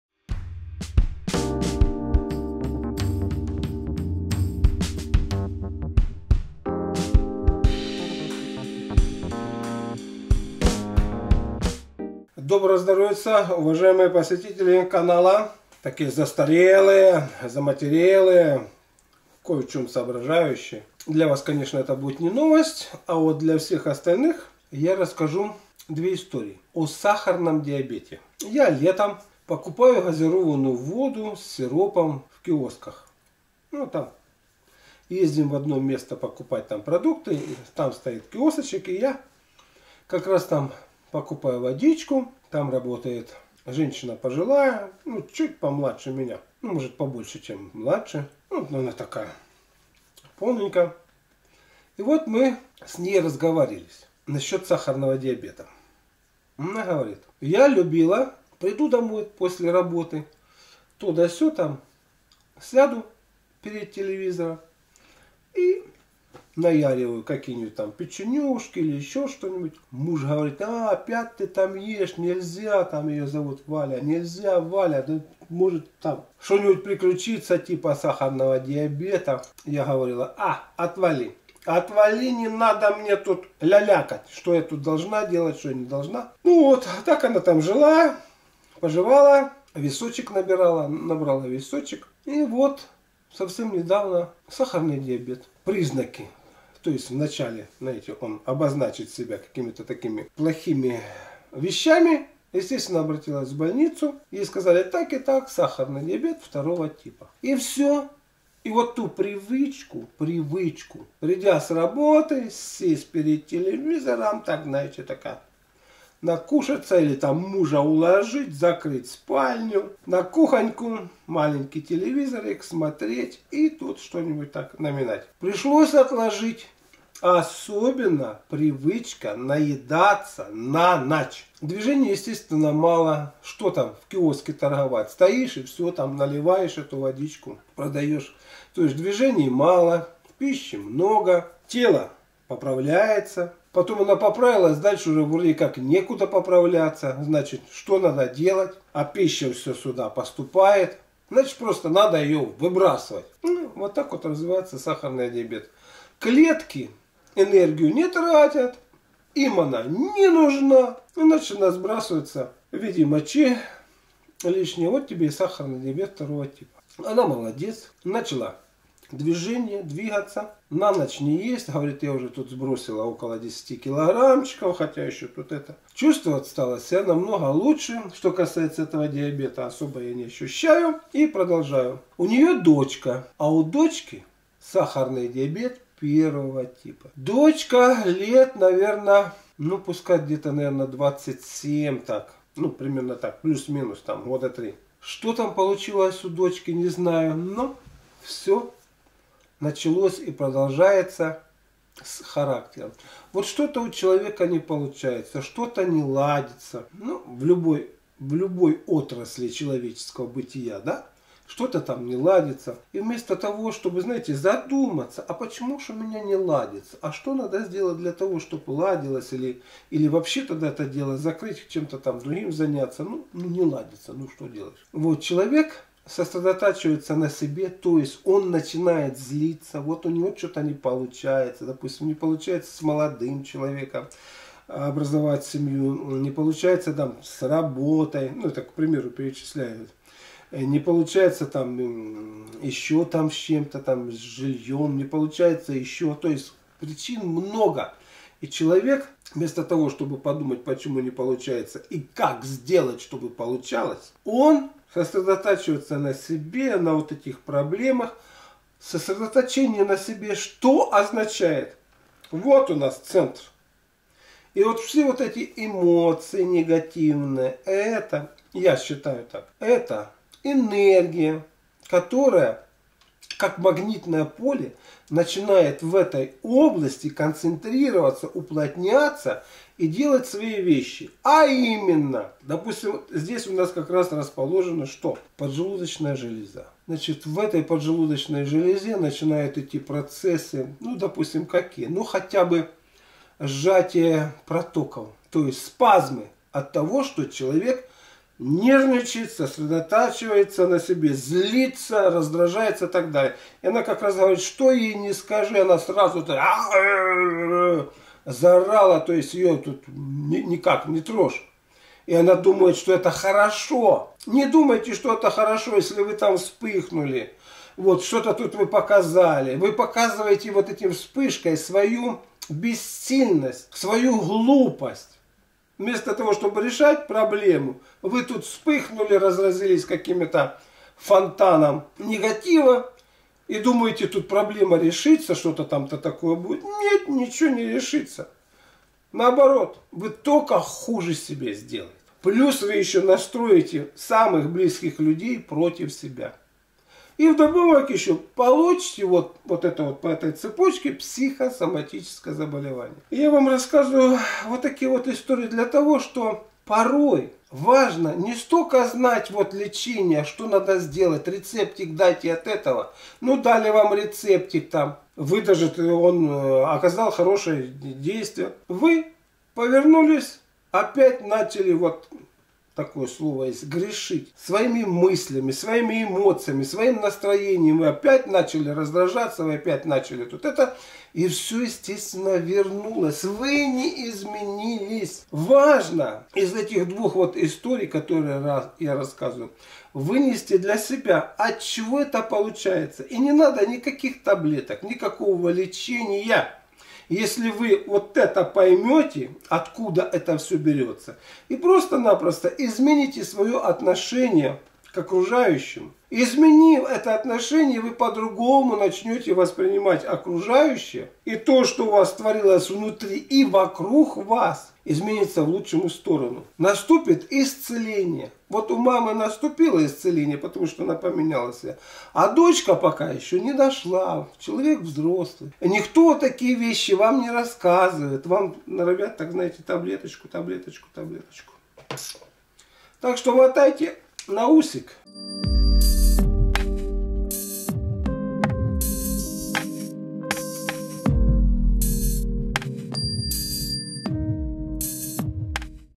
САХАРНЫЙ ДИАБЕТ. ДВЕ ИСТОРИИ. Малахов.
Сахарный диабет … как он наживается и как с ним можно работать! Поучительный опыт двух женщин … мамы и дочери! Одна ела и сидела, другая … злились и кричала!